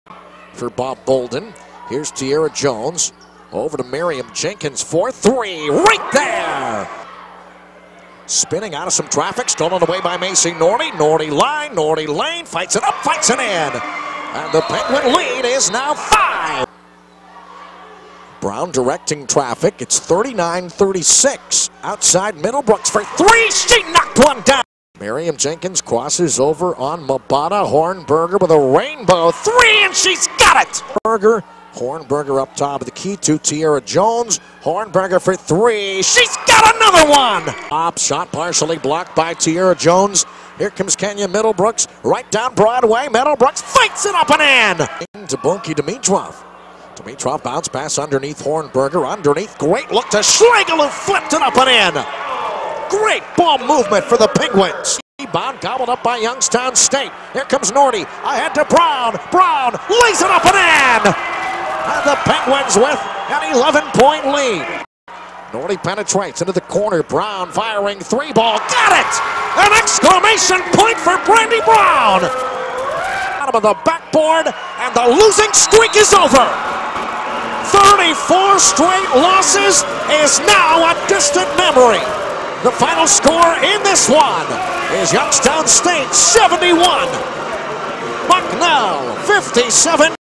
YSU Women's Basketball Highlights vs. Bucknell | Nov. 24, 2010
Highlights from the Youngstown State women's basketball team's 71-57 victory over Bucknell on Nov. 24, 2010 at the Beeghly Center in Youngstown, Ohio.